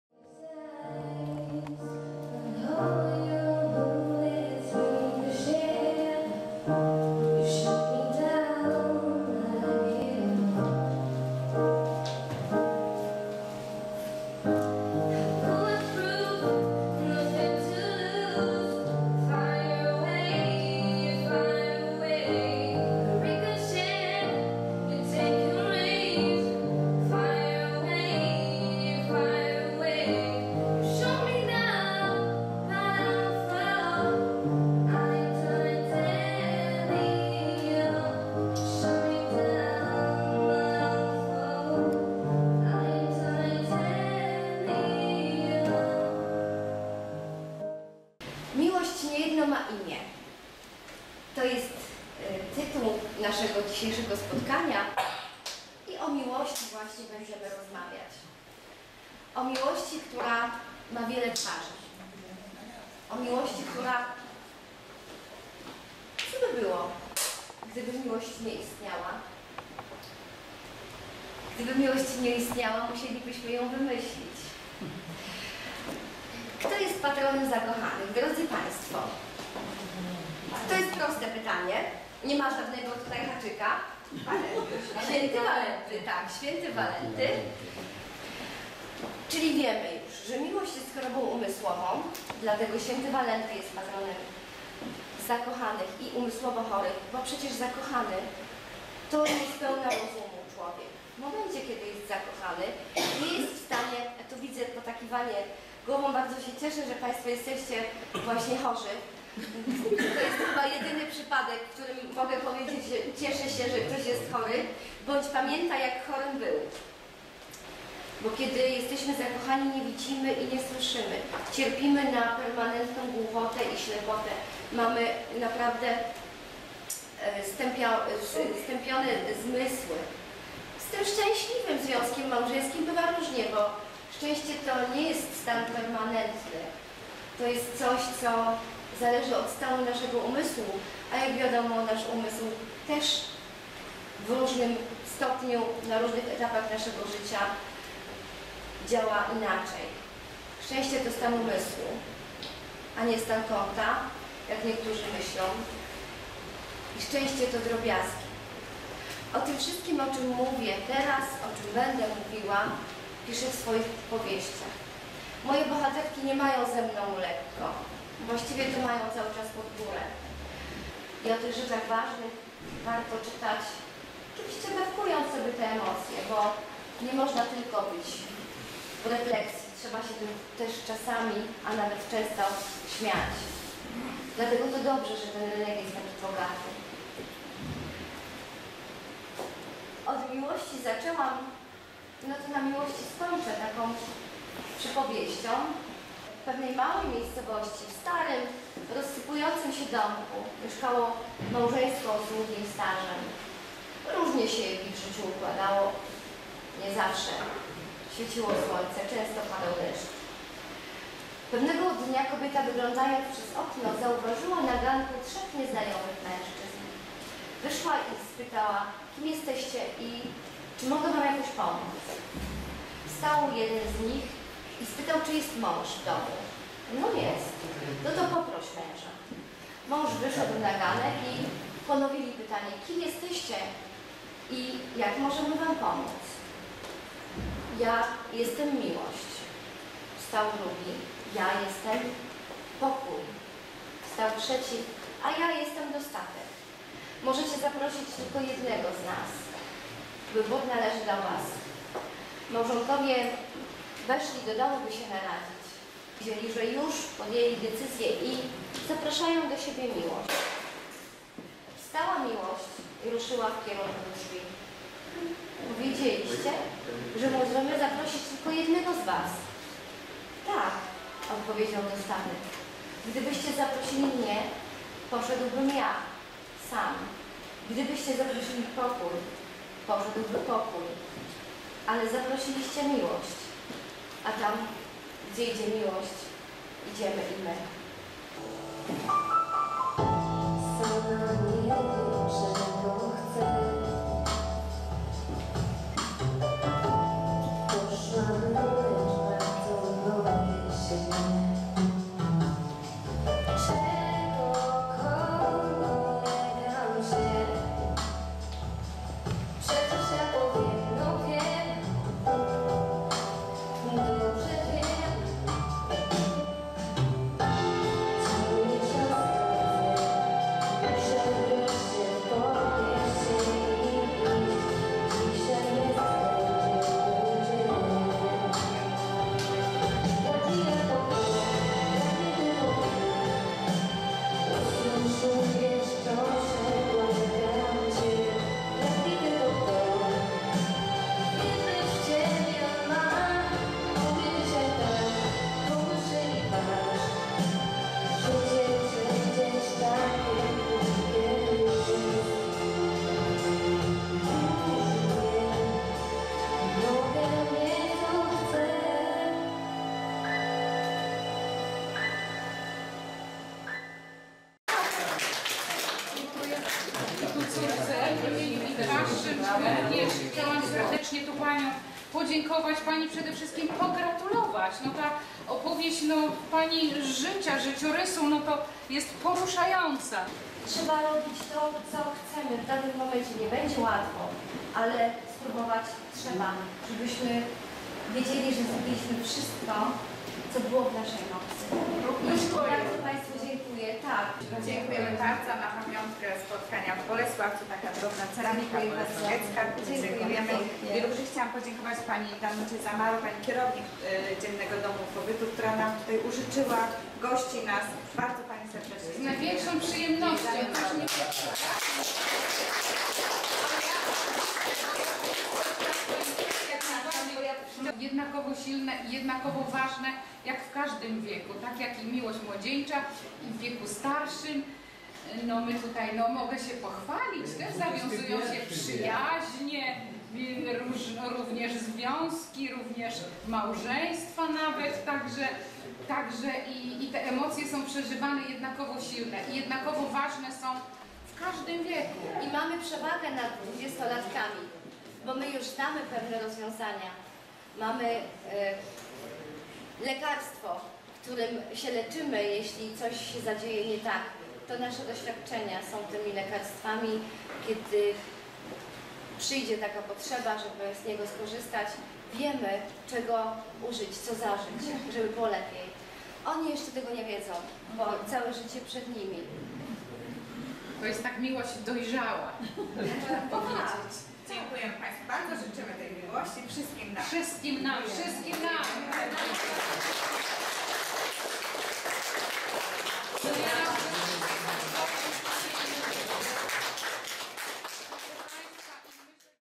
Monika Sawicka Karolina Sawicka spotkanie autorskie i recital
Spotkanie autorskie i recital - filia nr 1, Dom Dziennego Pobytu - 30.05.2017